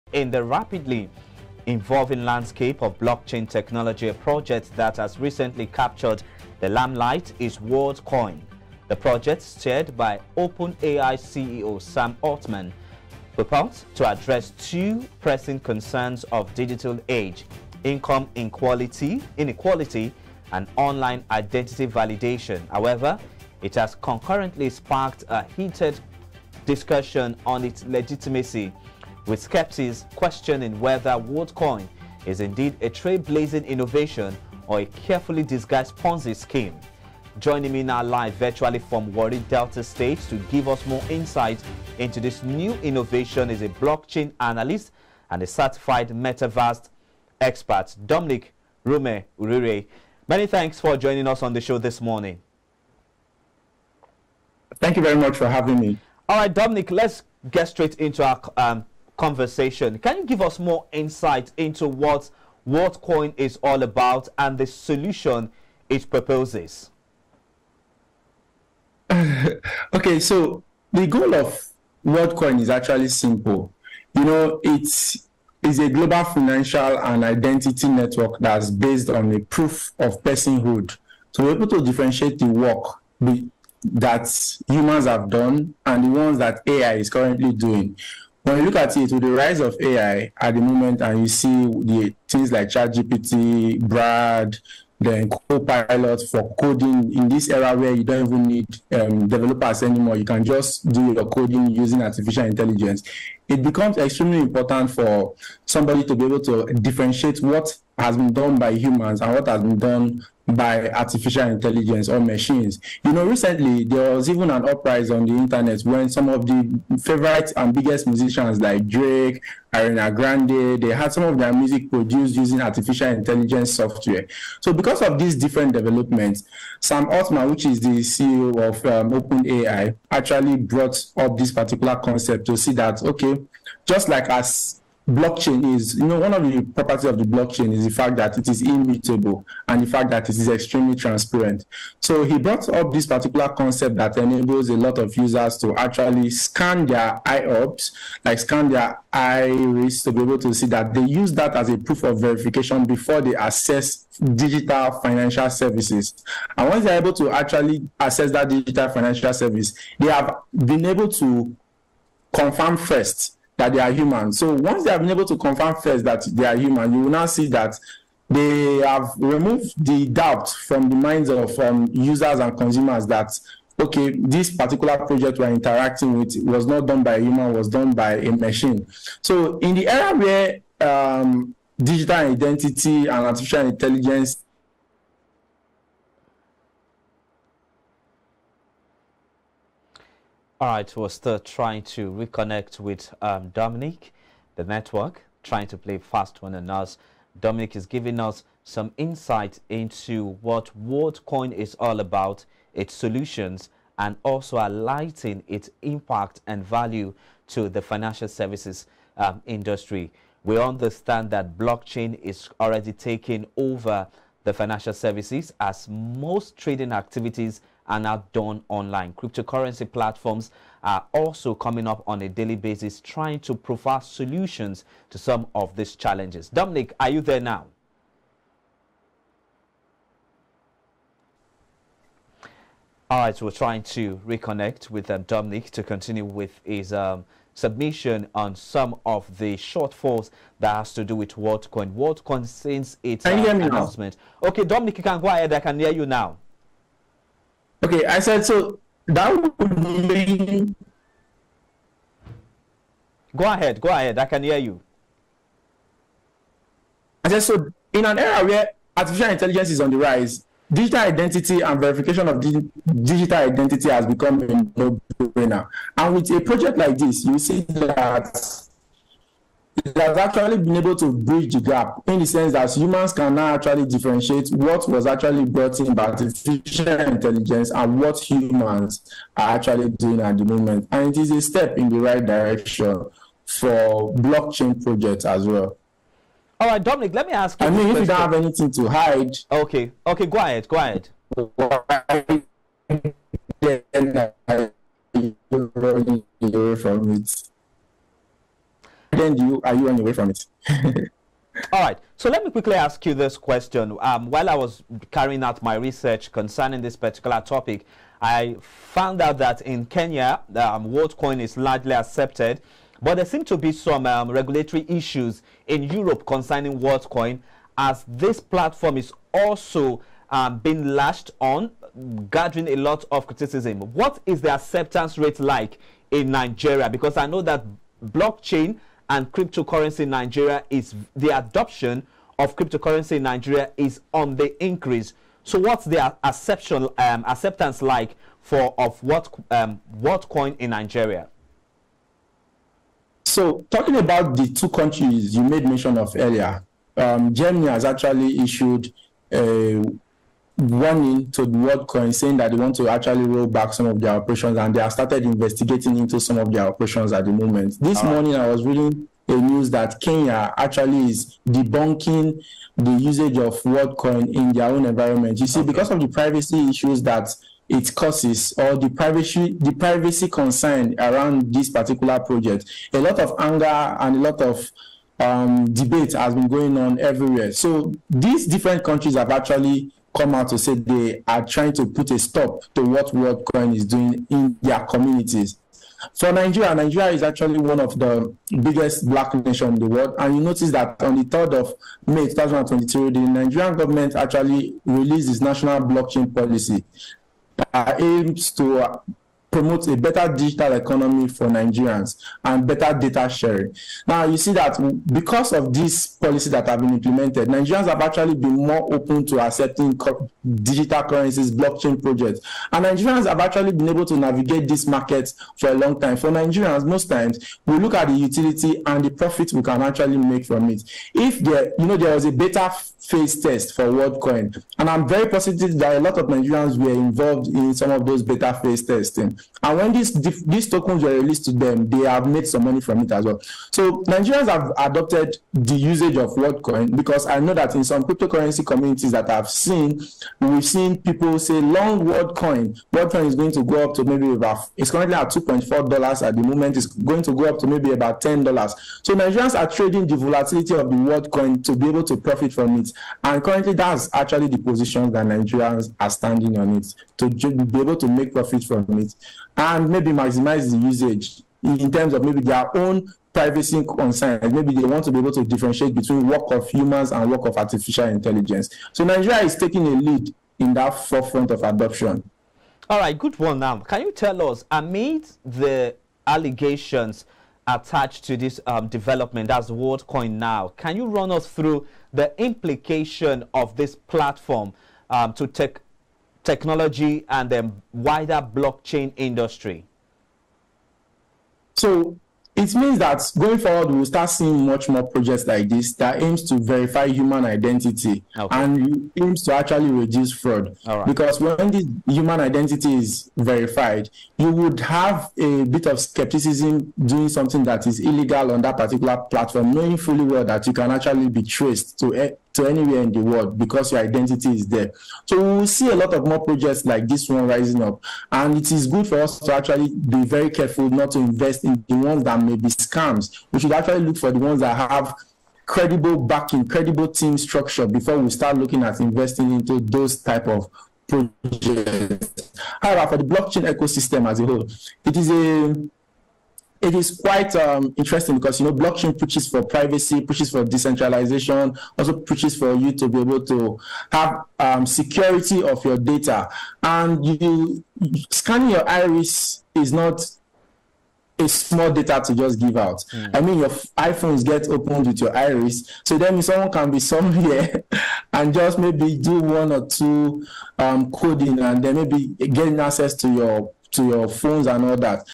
Digital Economy: Impact of Worldcoin On Blockchain Industry
The project founded by OpenAI CEO Sam Altman launched last week. It requires users to give their iris scans in exchange for a digital ID, and in some countries users also get free cryptocurrency as part of plans to create a new "identity and financial network".

This has raised concern whether Worldcoin is a digital coins or a ponze scheme.